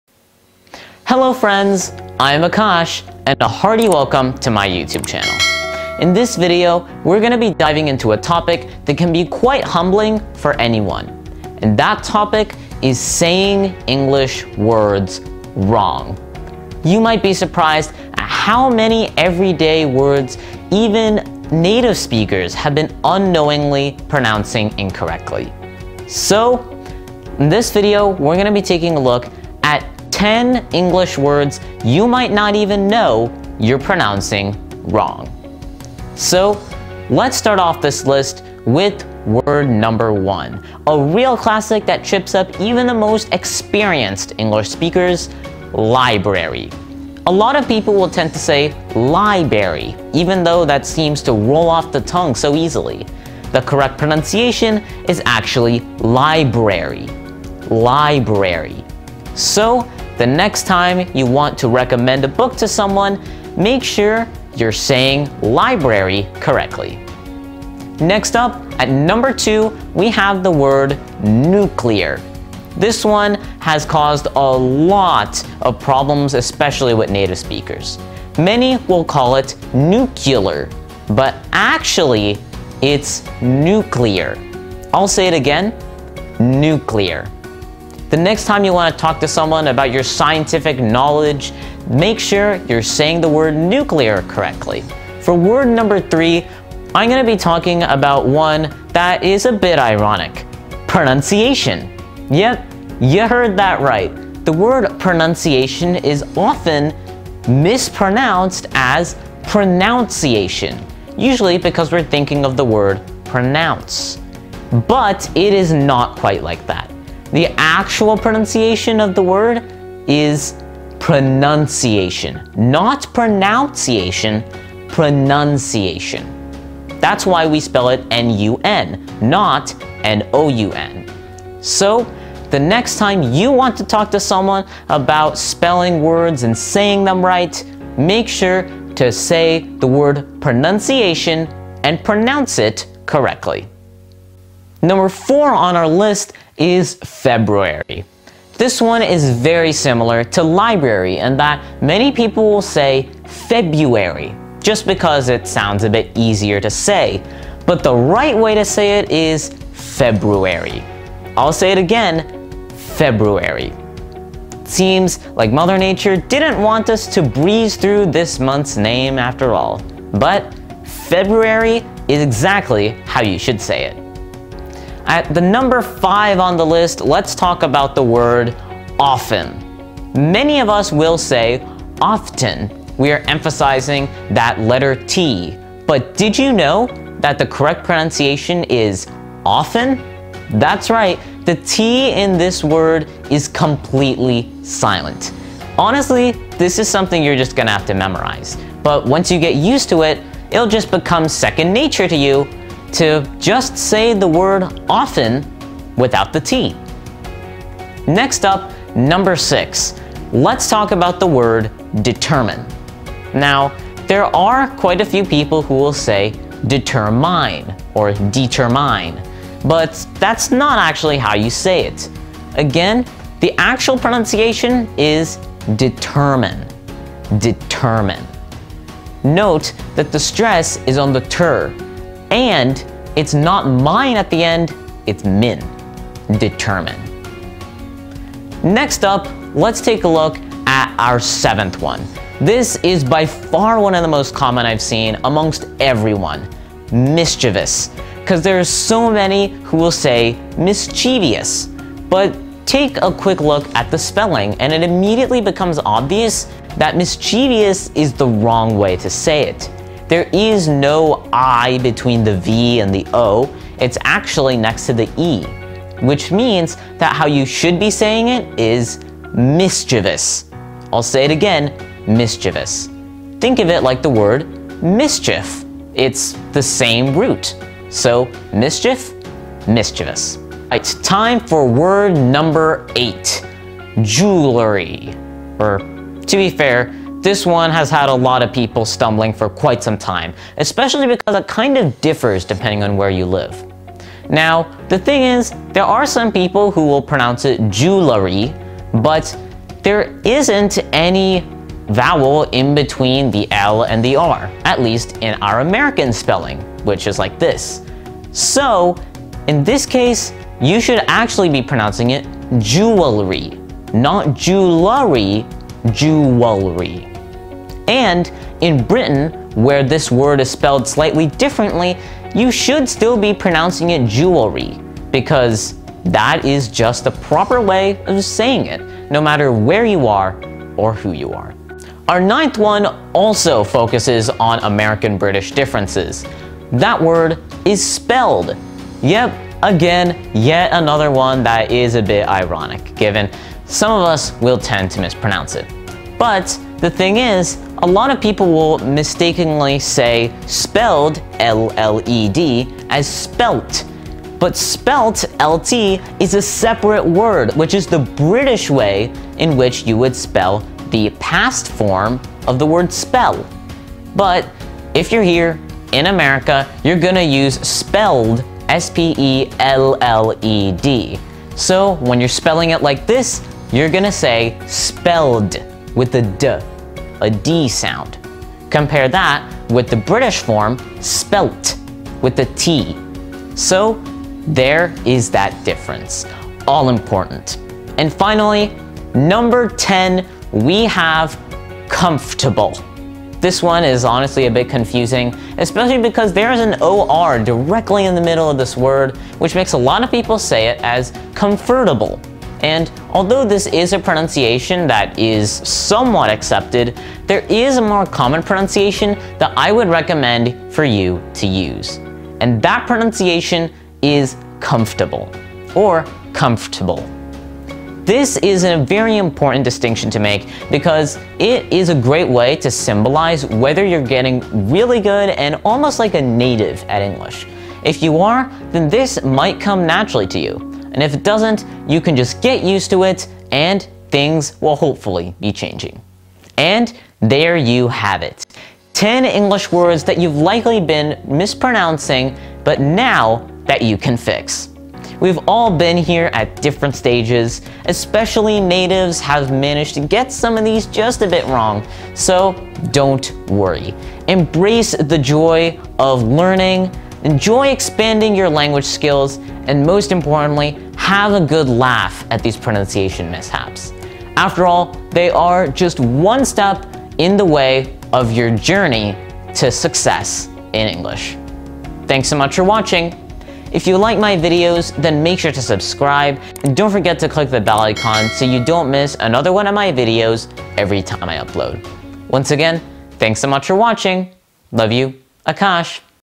10 English Words You Might Be PRONOUNCING Wrong | Akash Vukoti
You might be surprised at how many everyday words you’ve unknowingly been pronouncing incorrectly. In this video, I introduce 10 words that you might be PRONOUNCING wrong!